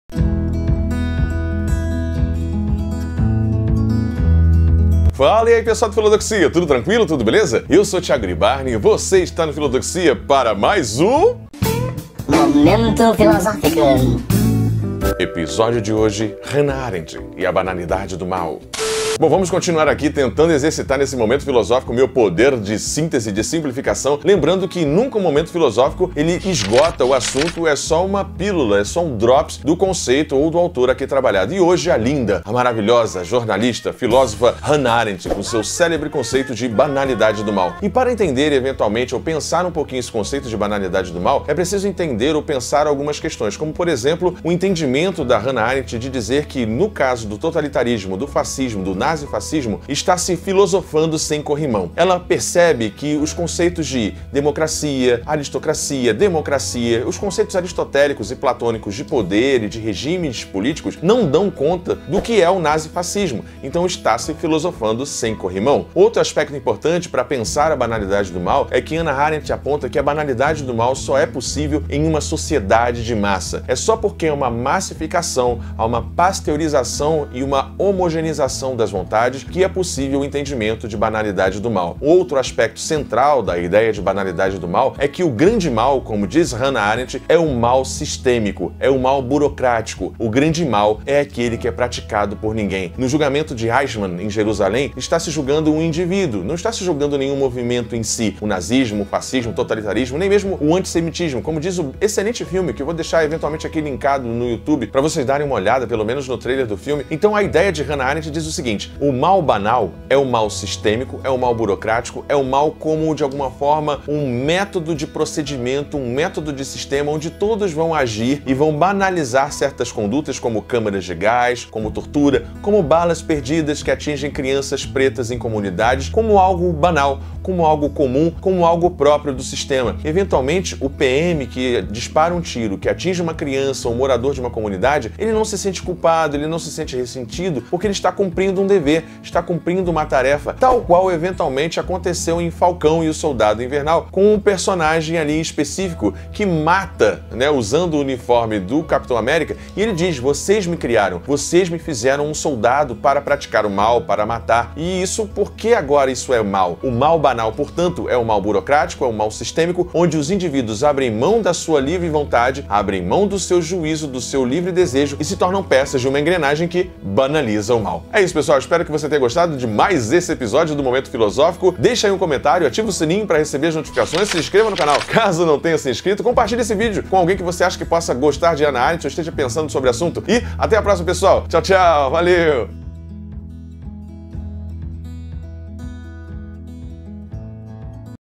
🦉HANNAH ARENDT: BANALIDADE DO MAL | MF #04
Hoje pensamos a potente filosofia de Hannah Arendt e o seu "filosofar sem corrimões"; está pronta(o)? Então...Simbóra!
Vale lembrar que esse breve vídeo não substitui seus estudos e a devida leitura filosófica, blz?

✨Roteiro e apresentação: @filodoxia_prof.iribarne
Edição: @talesbarradas